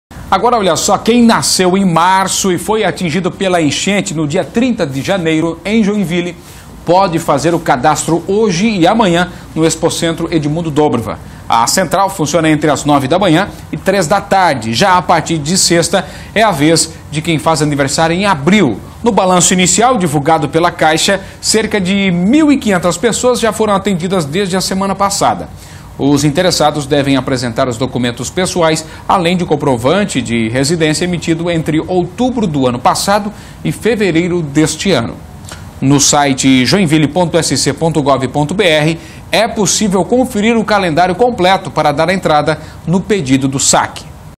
Inicia cadastro para liberação do FGTS de quem nasceu em março e foi atingido pela enchente
Quem nasceu em março e foi atingido pela enchente no dia 30 de janeiro, em Joinville, pode fazer o cadastro hoje e amanhã no Expocentro Edmundo Doubrawa.